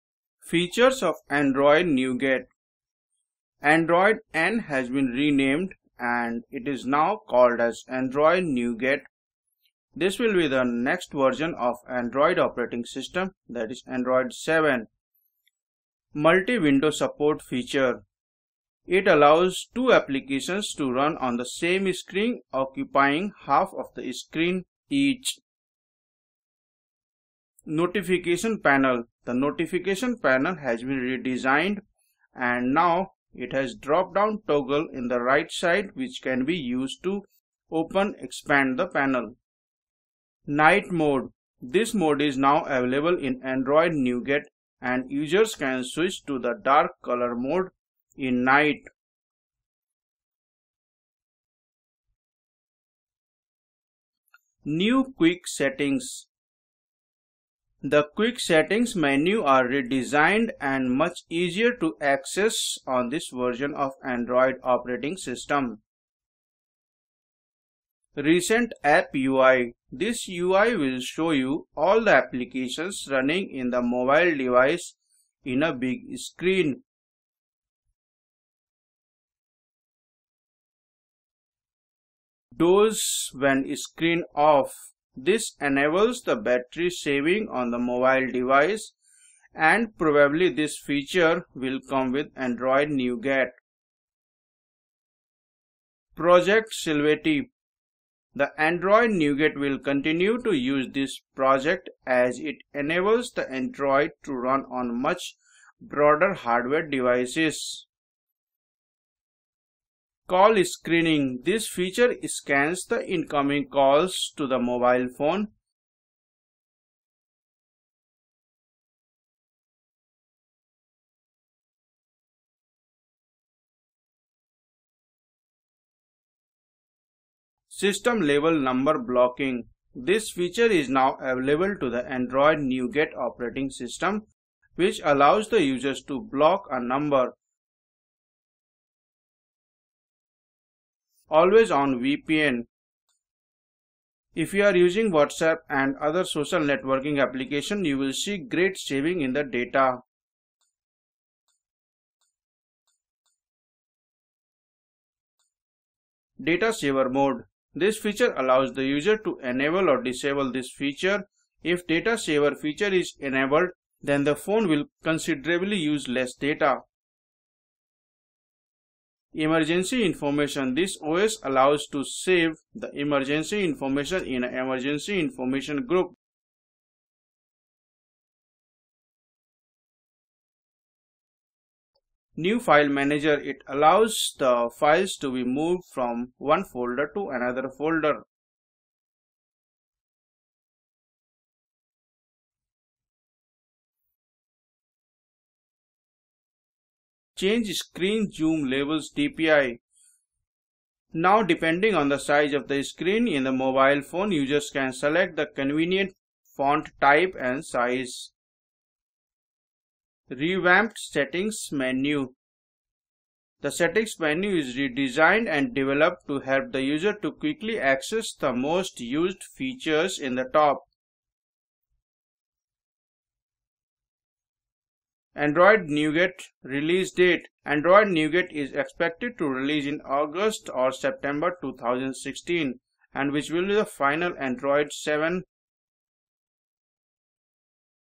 Features of Android Nougat
In this video we are exploring the features of Features of Android Nougat. Next version of Android Operating system is named as Android Nougat and the version of Android will 7.

Most important feature of Android Nougat is its ability to run two application on the same screen side by side. This very good feature of mobile phone and now it is available on the system level in Android.